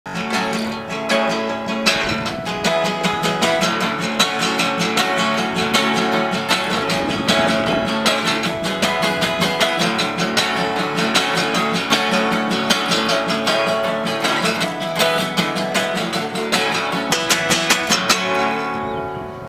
The Trouble With Templeton bei FluxFM: Warmspielung zum Morningshowcase
Kurz vor dem Auftritt beim "MorningShowcase am Nachmittag" (am 15. Mai 2014) spielen sich The Trouble With Templeton schonmal auf der FluxFM-Dachterrasse warm!